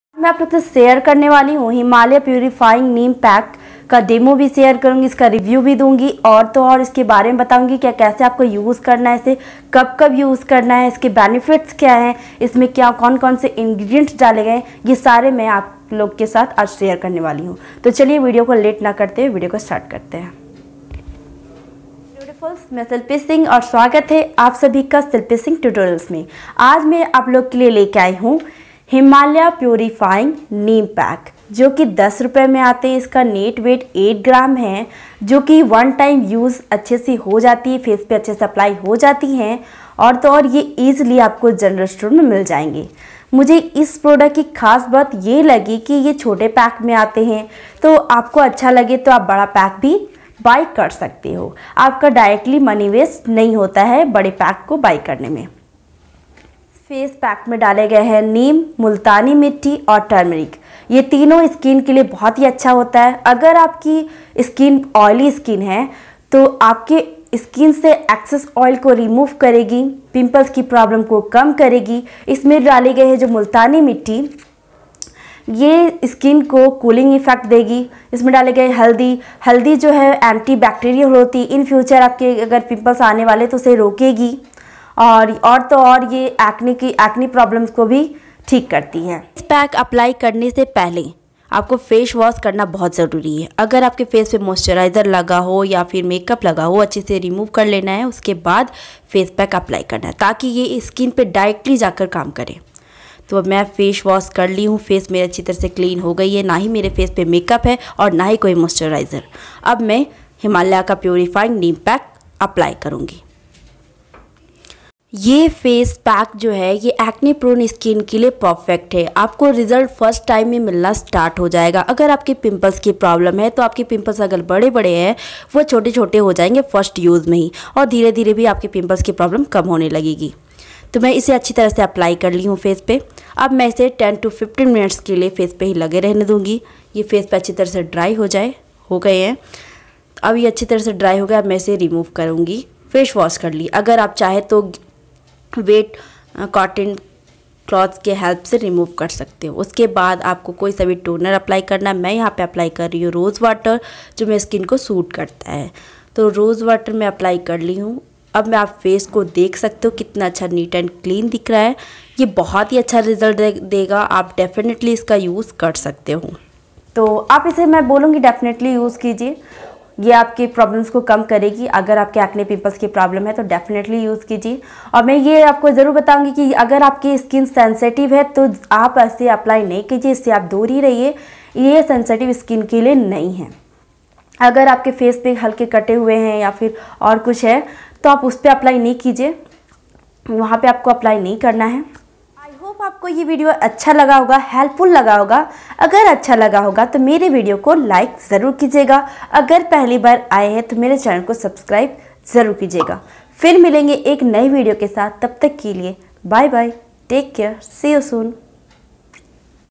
Himalaya Purifying Neem Face Pack Full Details | How To Use - Apply - Review - Result
In This Video I Am Going To Share With You

Any Queries Please Ask On Comments As Well Tag Me On Insta @shilpisingh8804